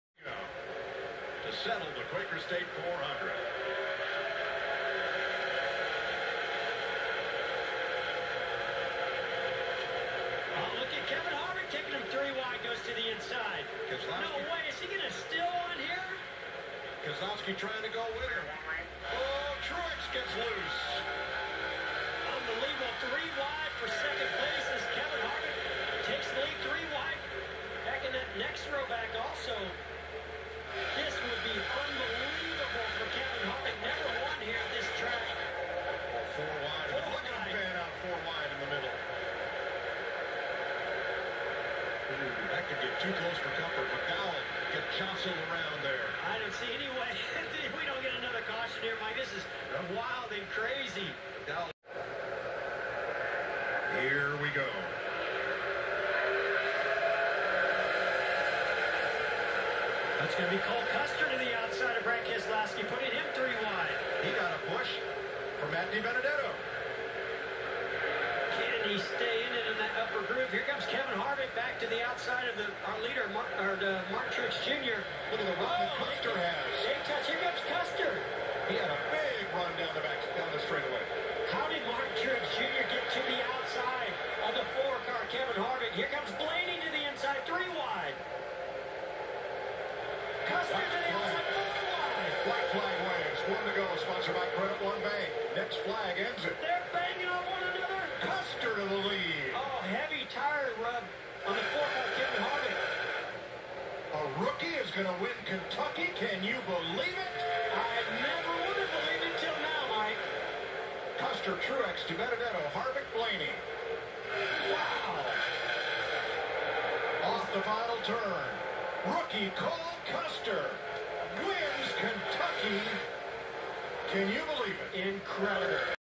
Kevin Harvick #4 Last Two Restarts @ Kentucky Speedway Teammate #41 Cole Custer Wins 2020 🏁
Kevin Harvick #4 @ Kentucky Speedway 😎 Teammate #41 Cole Custer Wins 🏁
(No Copyrights For The Video)
 ( Non-Profit Videos)